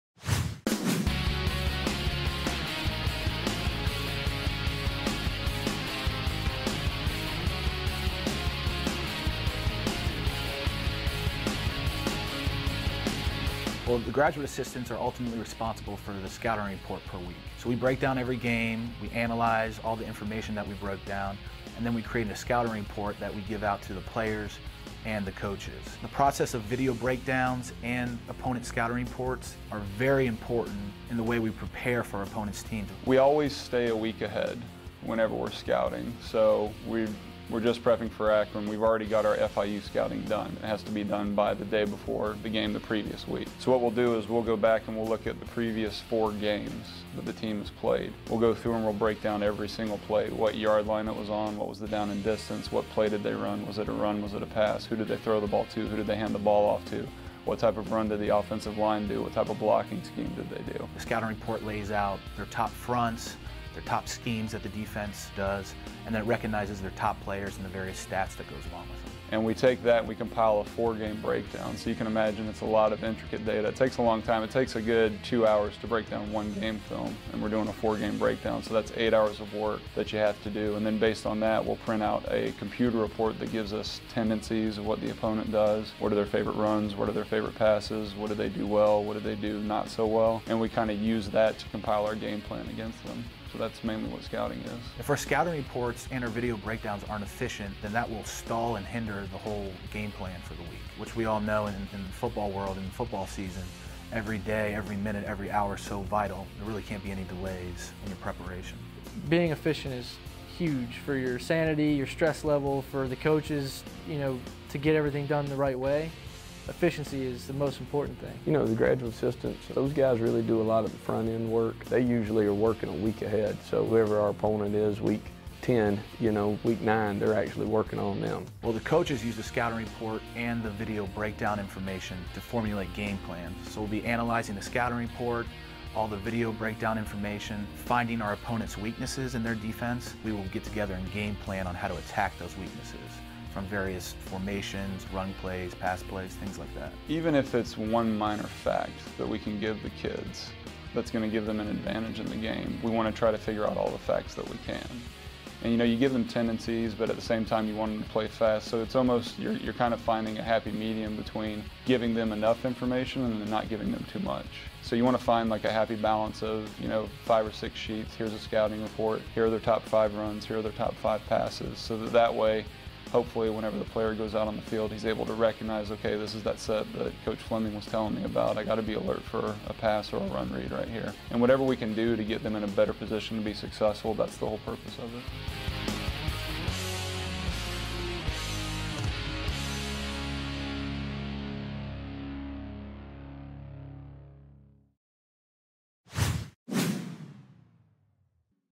Building the Scouting Reports for UCF Football
What does the opponent run on 3rd down and short off of the right hash within the tight red zone?  Check the scouting report.  "Inside UCF Football" goes in depth on how UCF prepares for their opponents, breaking down film, creating statistical analysis reports, and ultimately generating scouting reports. These reports help the coaches build the game plan and help the team focus on what they need to know for the next game to be successful.

Features interviews with: Defensive GA Paul Carrington,  Offensive GA Keegan Kennedy, Defensive GA Andrew Dodge, and Linebackers Coach Tyson Summers.

Charge On!